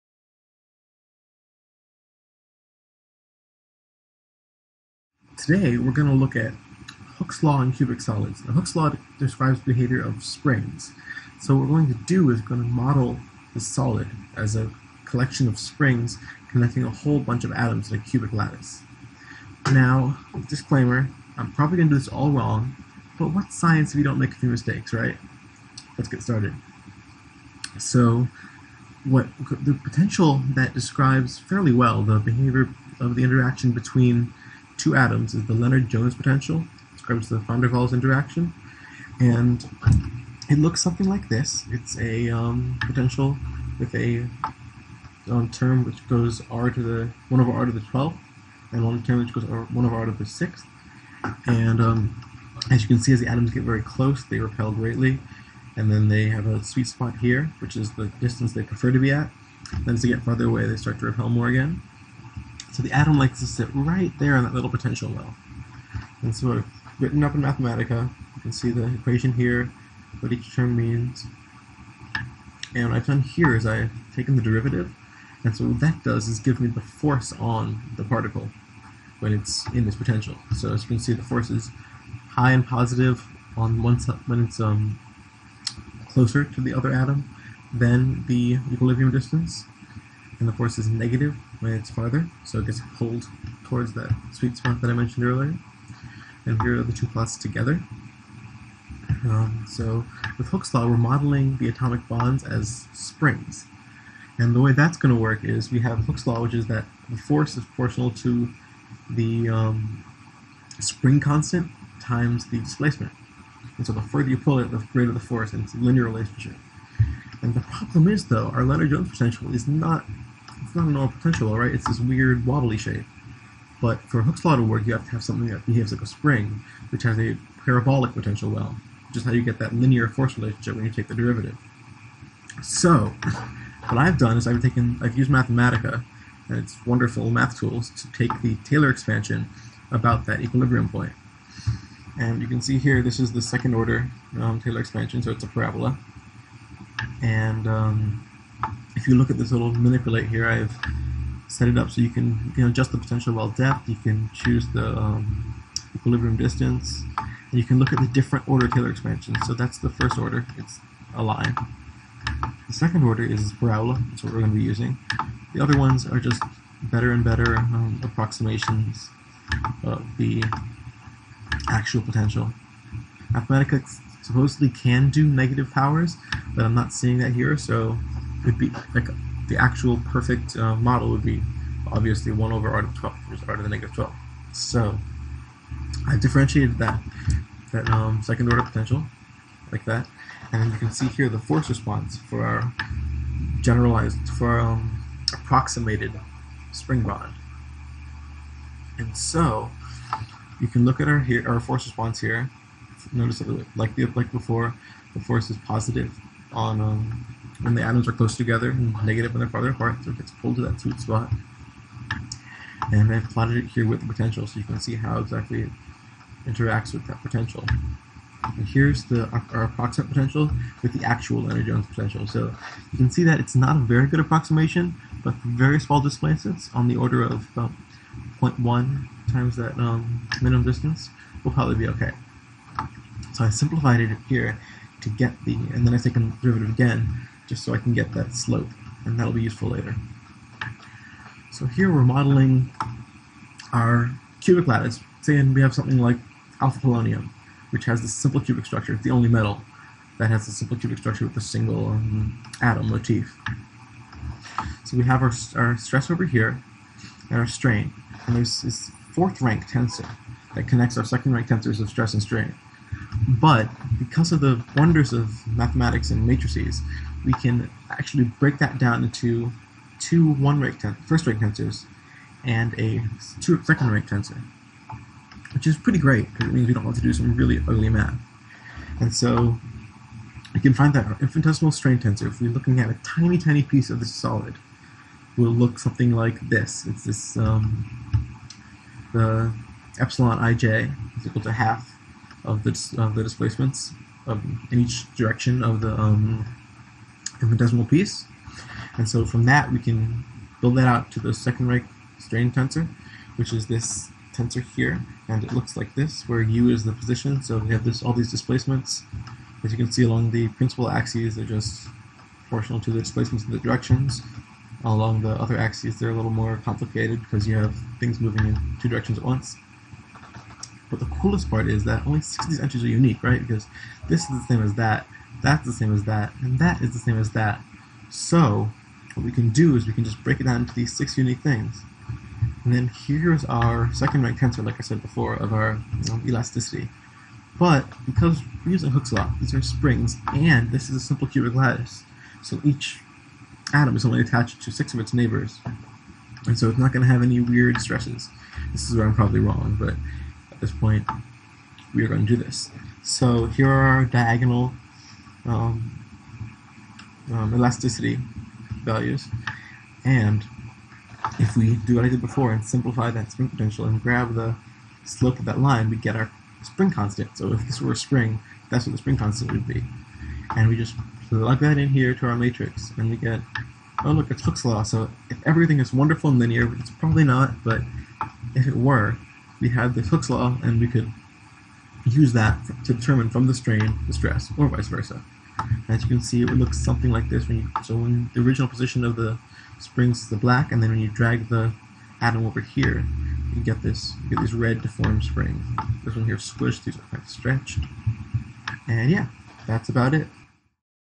Student Video: Hooke's Law in Cubic Solids
MIT RES.3-004 Visualizing Materials Science, Fall 2017
Speaker: anonymous MIT student

Describes the behavior of springs using Hooke's Law.